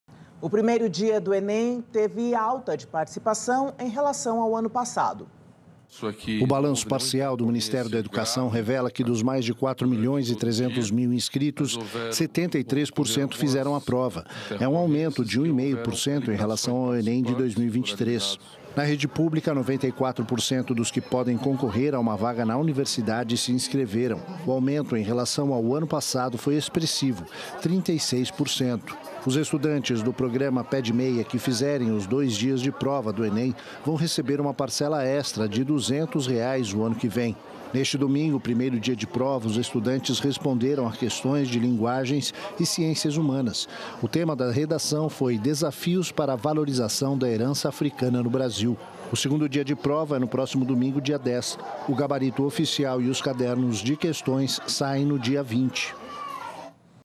ENEM: alta de participação marca o primeiro dia de provas
O balanço parcial do Ministério da Educação revela que, dos mais de 4 milhões e 300 mil inscritos, 73% fizeram a prova. É um aumento de um e meio por cento em relação ao ENEM de 2023.

Na rede pública, 94% dos que podem concorrer a uma vaga na universidade se inscreveram. O aumento em relação ao ano passado foi expressivo: 36%.

Os estudantes do programa Pé de Meia que fizerem os dois dias de prova do ENEM vão receber uma parcela extra de duzentos reais no ano que vem.